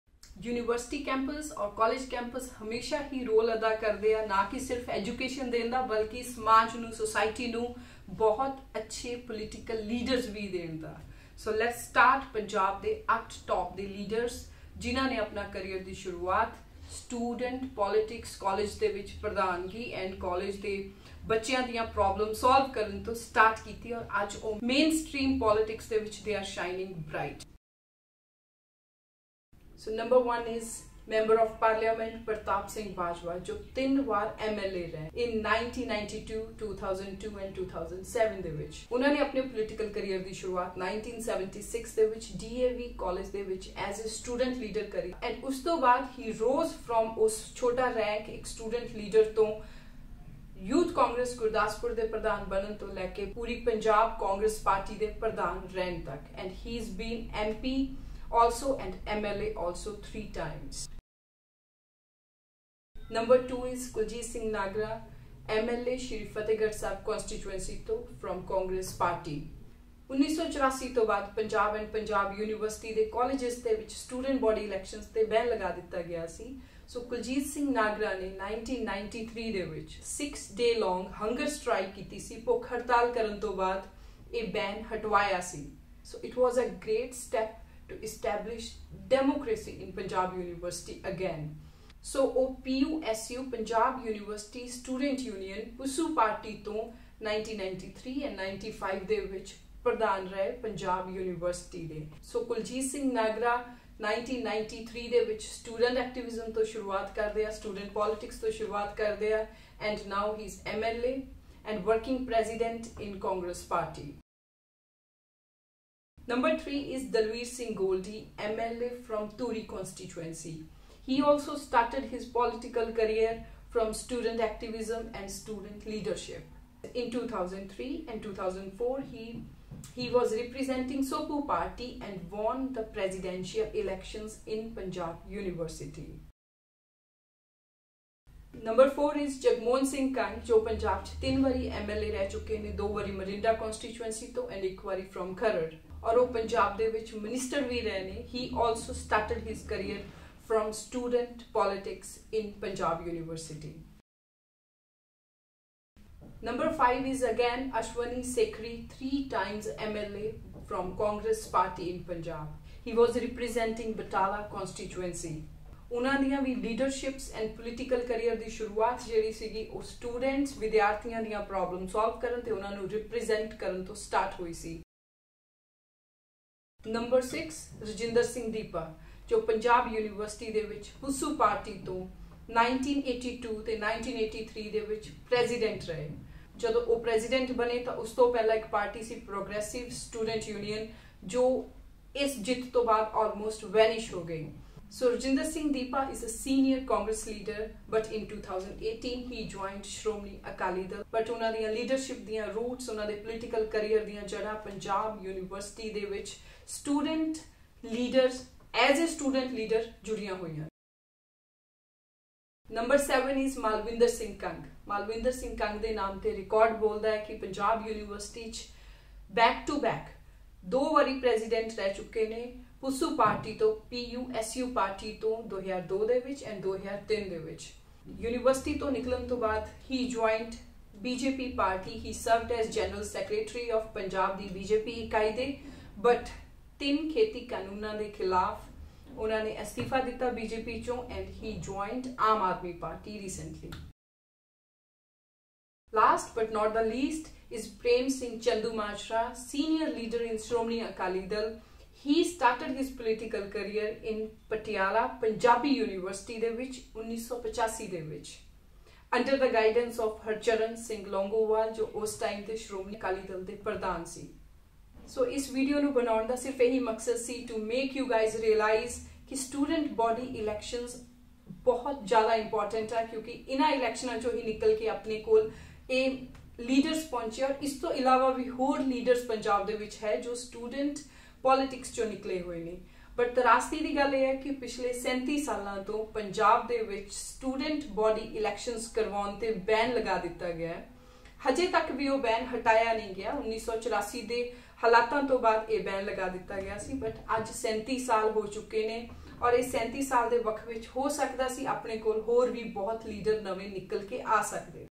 Top 8 Leaders in Punjab from Student Politics. ਕਾਲਜ ਦੀ ਪ੍ਰਧਾਨਗੀ ਤੋਂ ਪੰਜਾਬ ਦੀ ਵਿਧਾਨ ਸਭਾ ਤੱਕ ।
Watch this video to know top 8 punjabi leaders who emerged from student politics.